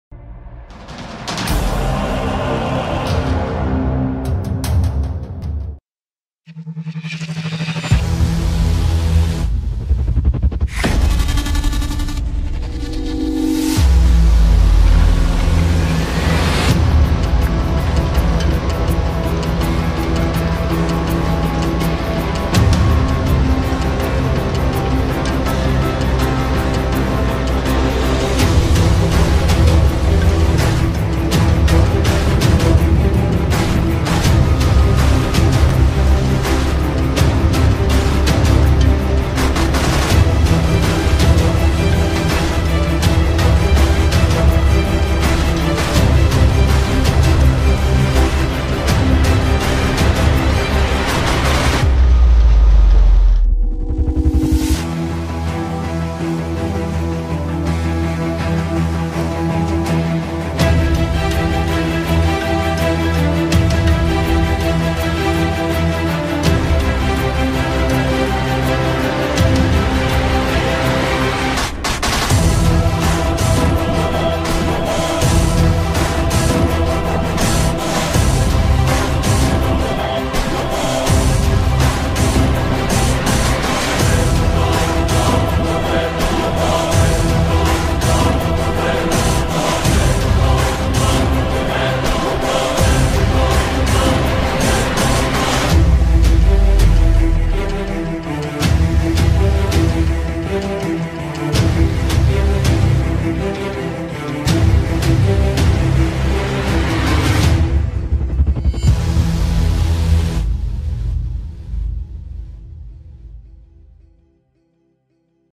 Holmer T3/ Arrachage betteraves / Agrividéo 77
Holmer T3 à l'arrachage de betteraves 2018, au transport un John Deere 6810 !

Agrividéo 77/ Octobre 2018/  614 abonnés